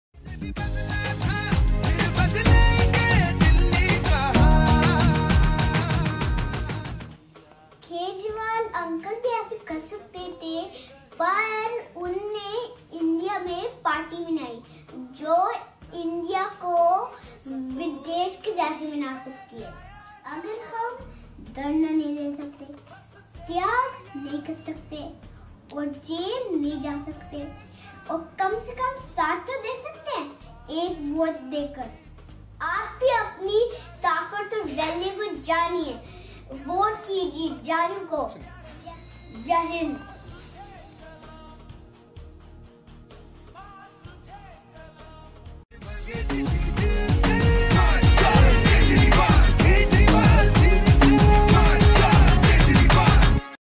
What we can do to help Kejriwal?
In this video A child explains How we can help Kejriwal?